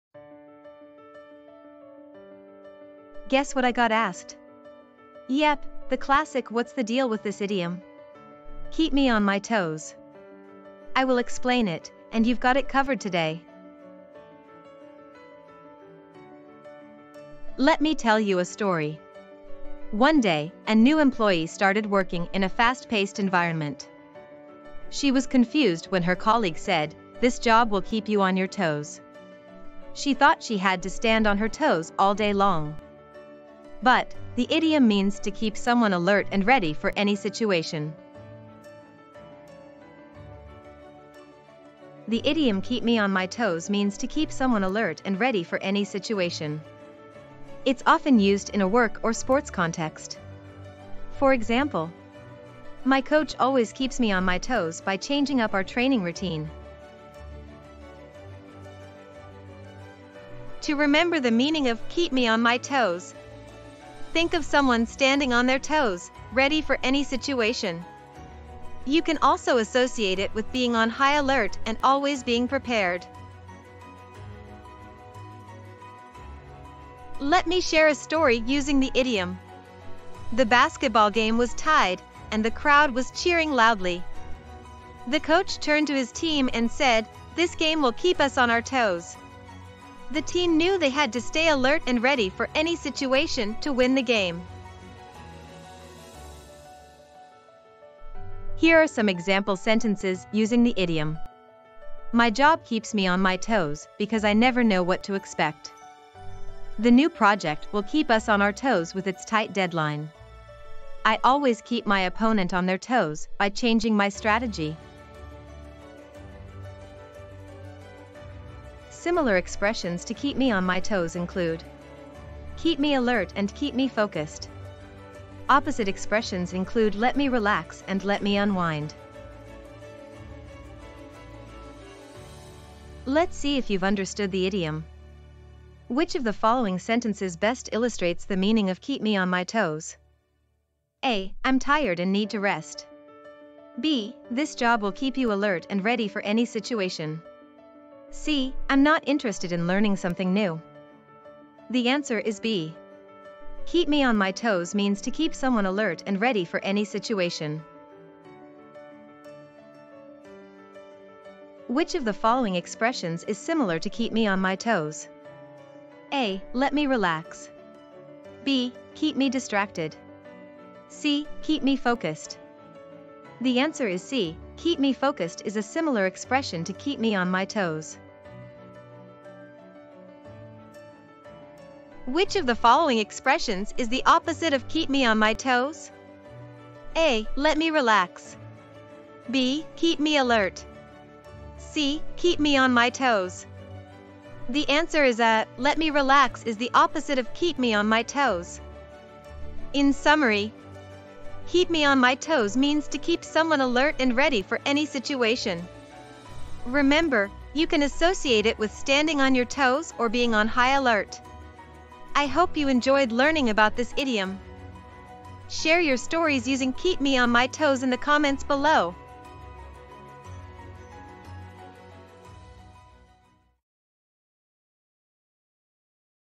'Keep Me on My Toes' Explained in Detail | English Idiom Lesson
Hey there, language learners! Today, we're unraveling the meaning behind "keep me on my toes."

0:00 Introduction
0:14 Definition
0:57 Memory Aid
1:15 Storytime
1:35 Usage Examples
1:56 Similar & Opposite Expressions
2:10 Quizzes
3:19 Summary